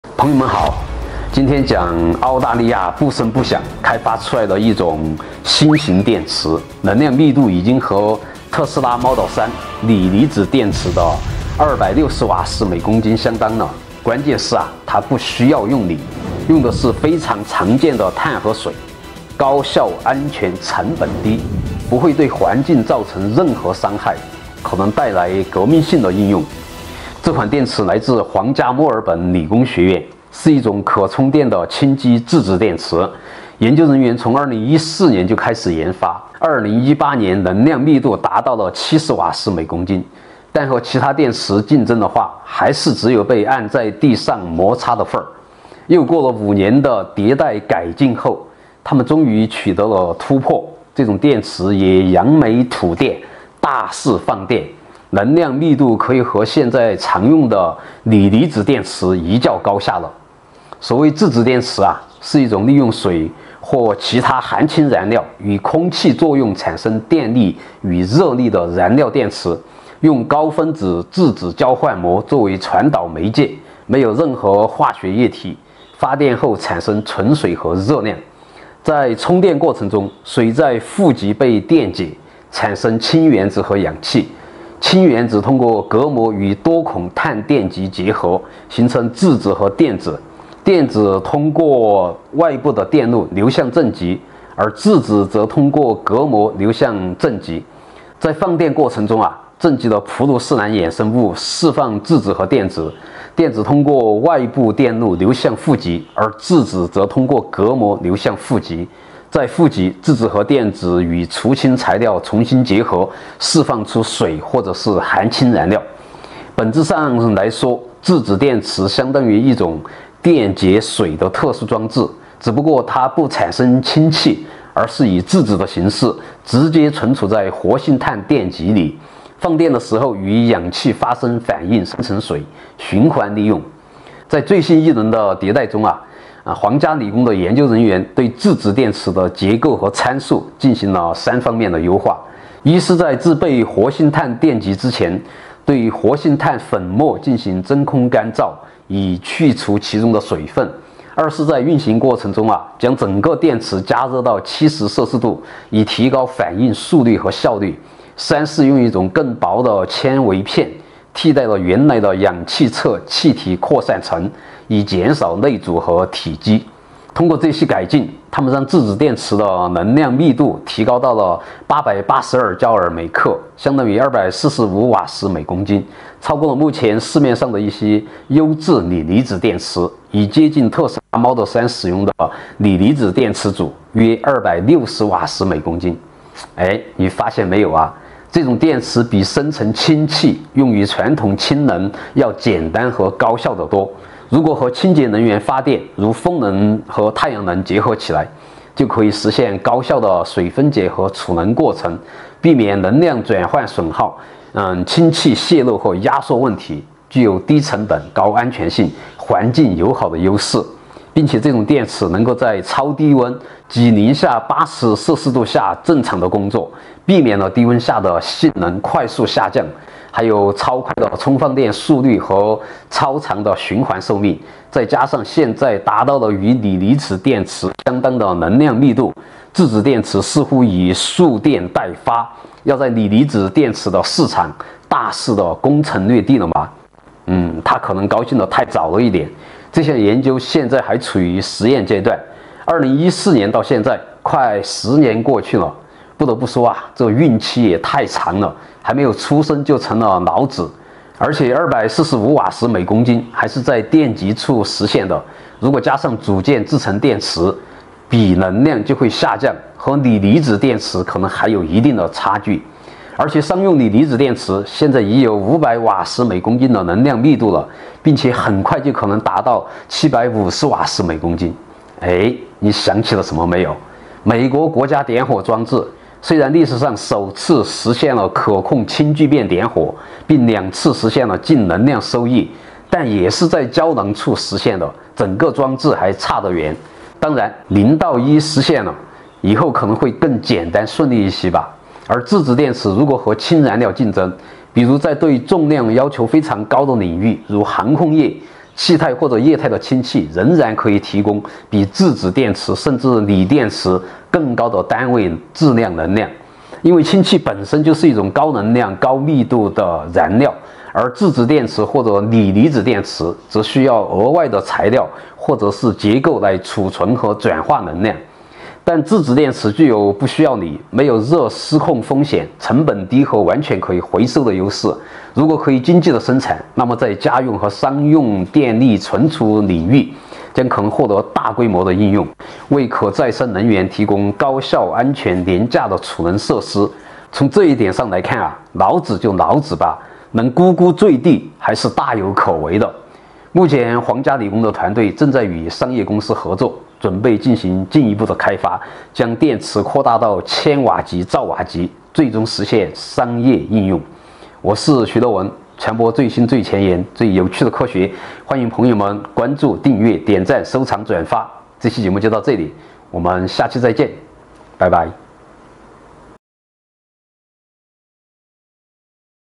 澳洲新電池不用鋰了！可媲美特斯拉鋰電，或帶來革命性突破
今天講澳大利亞不聲不響，開發出來的一款新型電池，能量密度已經和特斯拉Model 3，鋰離子電池的260Wh/kg相當了，關鍵是它不需要用鋰，用的是非常常見的碳和水，高效安全成本低，不會對環境造成任何傷害，可能帶來革命性的應用。